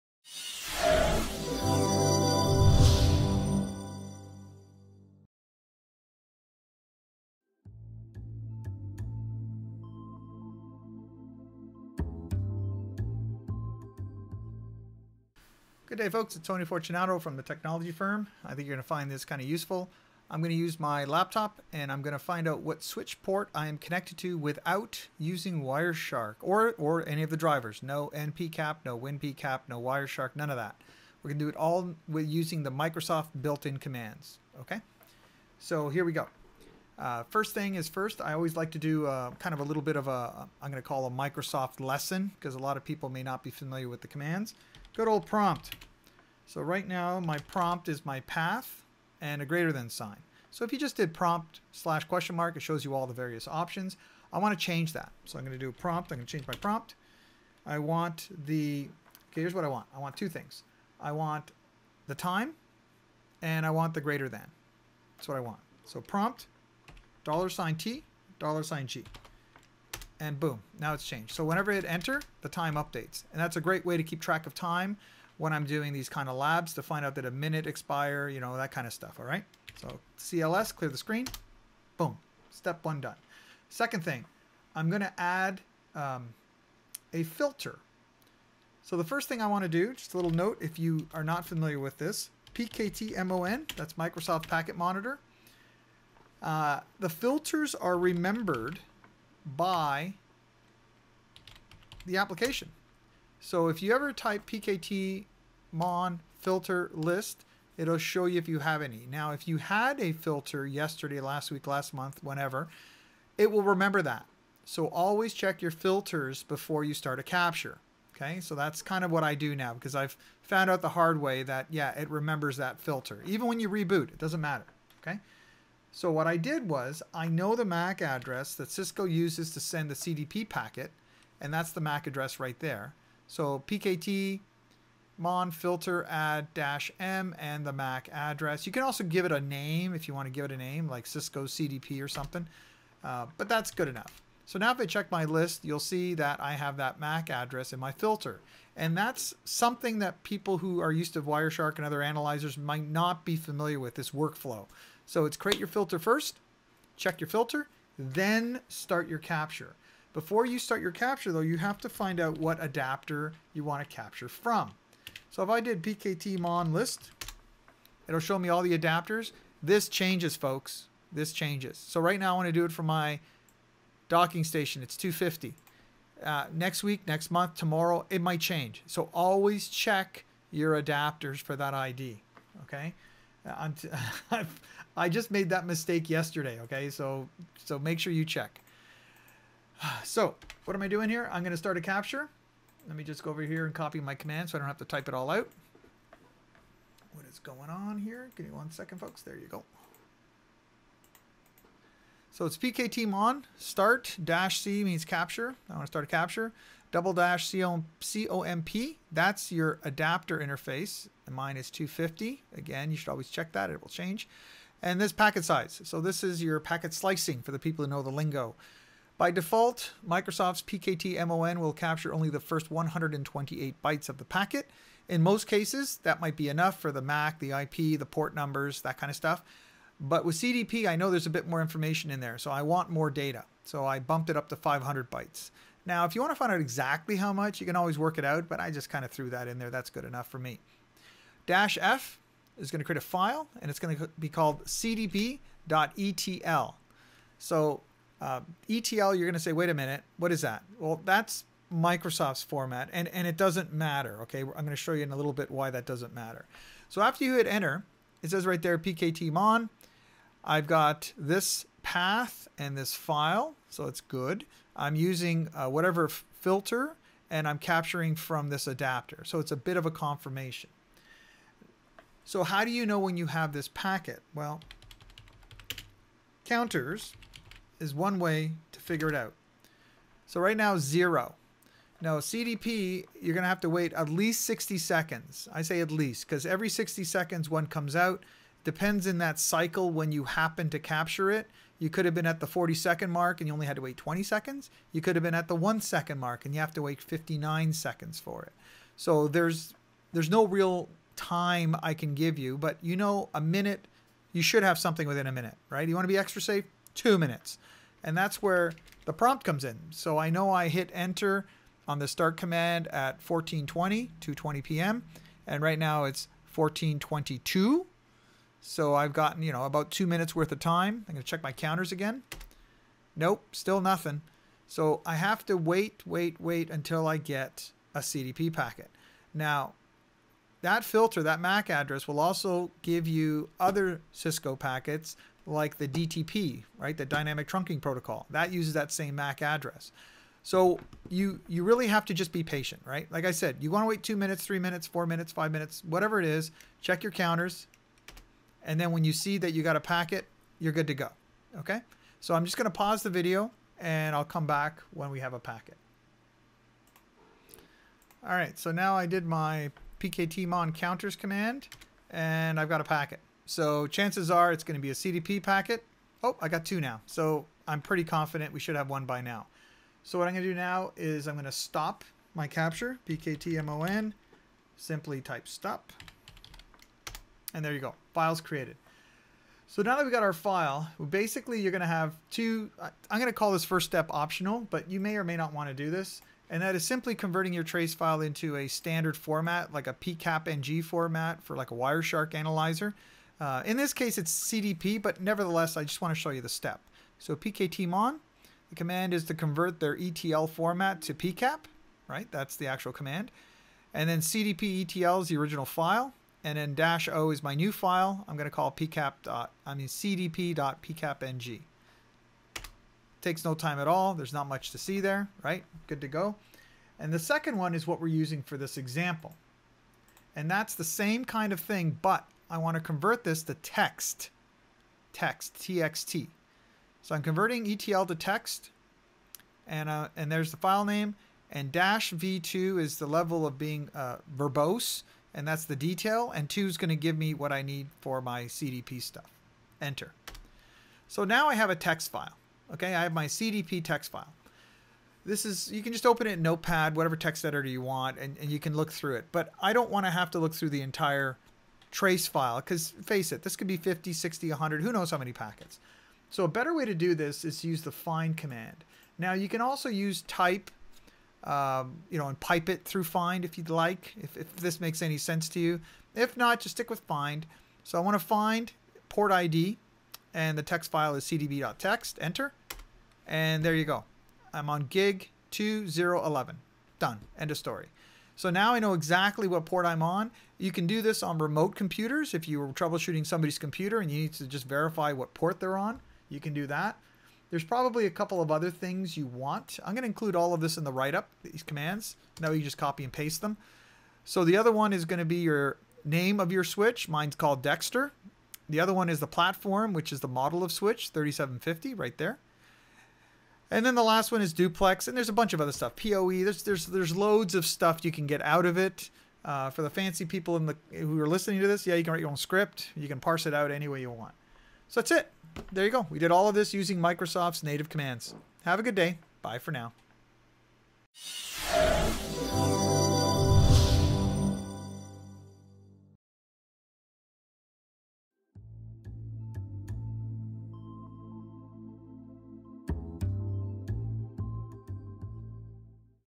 CDP Discovery Using Microsoft’s PKTMON
In previous articles I cover some of what I consider troubleshooting best practices and got back some really helpful feedback.

One person asked me “is there a way to figure out what port I am connected to without using Wireshark?”.  Wow what a great question.  Challenge accepted.

In summary, heres what I did; used Windows pktmon command to capture only packets addressed to Cisco’s multicast mac address, convert the trace to a text file, then use Microsoft’s find command to get the info I needed.

Here are a list of the commands I used in the video for your reference.
prompt $G$T$G
pktmon filter add -m 01:00:0c:cc:cc:cc
pktmon list
pktmon  start -c --comp 250 --pkt-size 500 -f cdp.etl